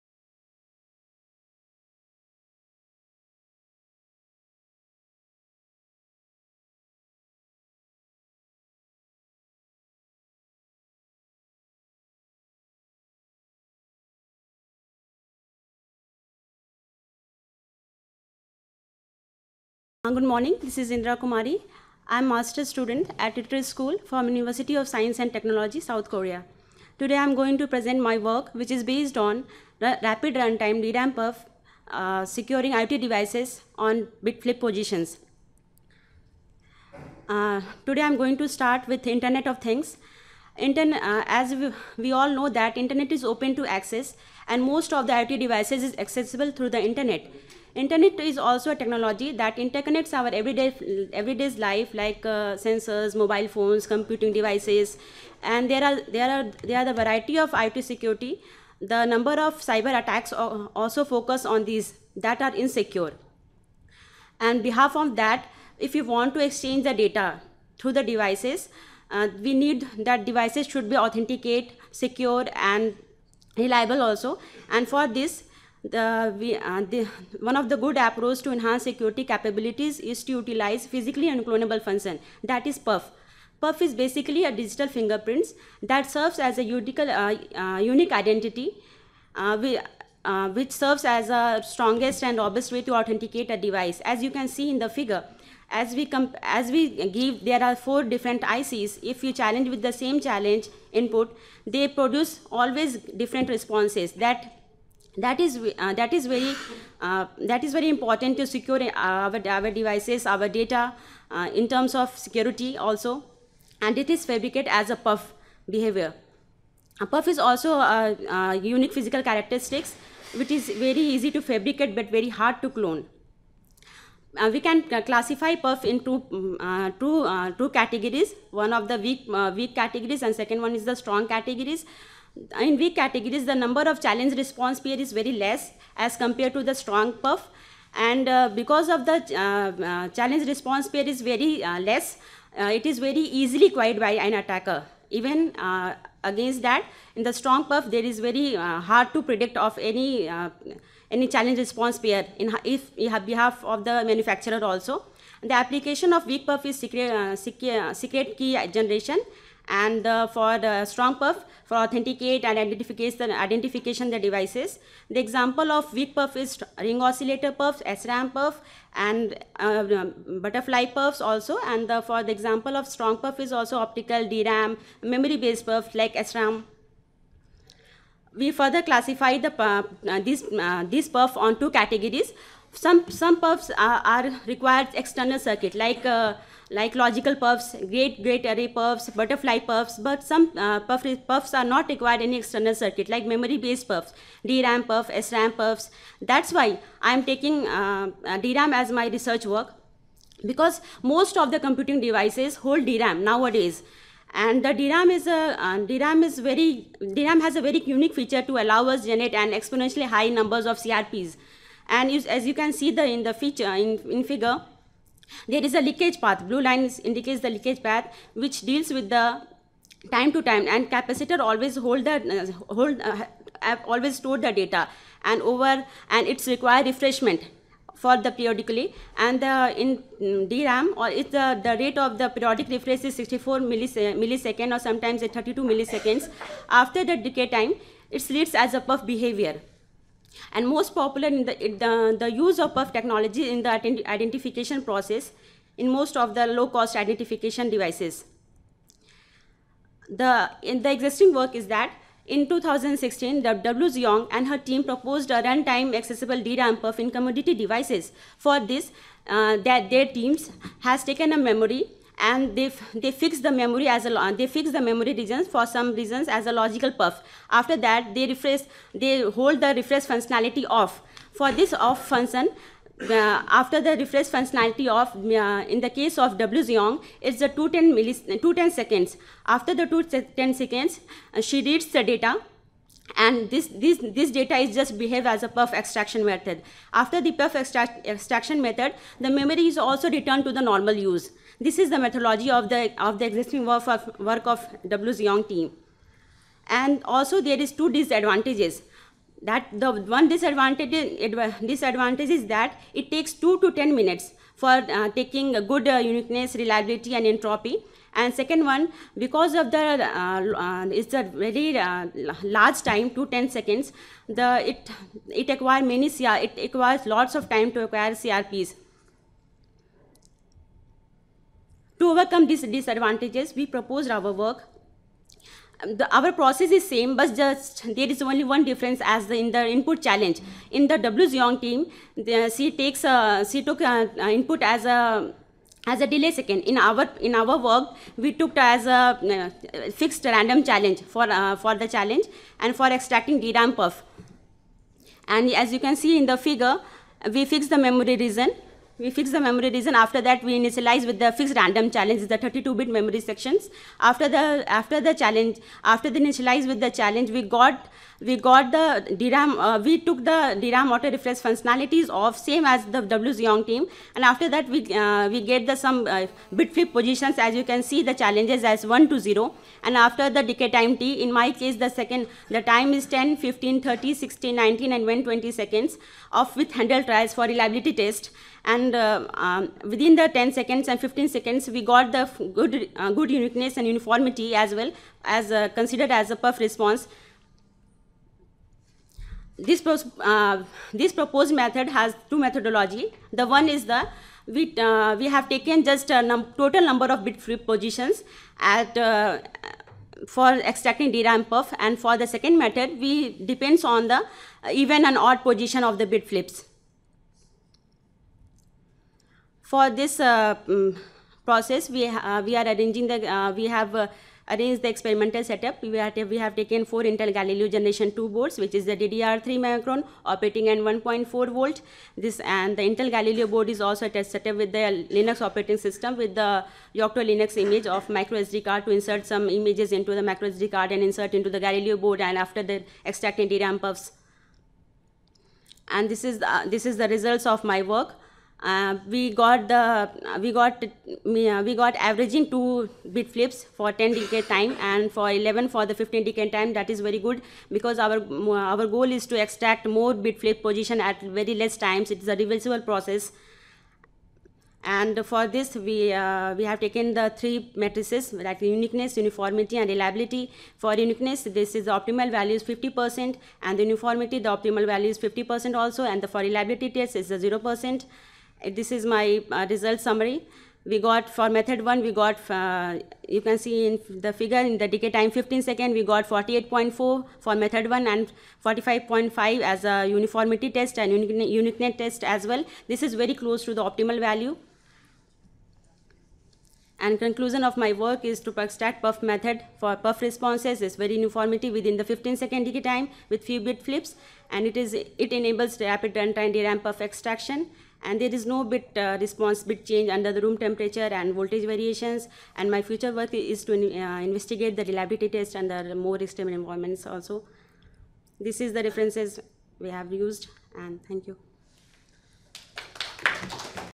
Rapid Run-Time DRAM PUF Based on Bit-Flip Position for Secure IoT Devices
Author:
Indra Kumari, Mi-Kyung Oh, Yousung Kang, Dooho Choi

Abstract:
IoT services are expanding beyond basic sensor networks to various services such as connected cars, smart factories, and smart cities. As a result, security issues are also growing rapidly, especially for low-cost IoT devices. One of good security solutions is to use the Physical Unclonable Function (PUF) technology to uniquely identify hardware components. In this paper, we propose a new approach to reduce the latency of the decay based DRAM PUF at run-time in commodity IoT devices. Experimental results based on Intel Galileo Gen2 Boards show that our methods can quickly generate the PUF values with good uniqueness and uniformity within 15 seconds.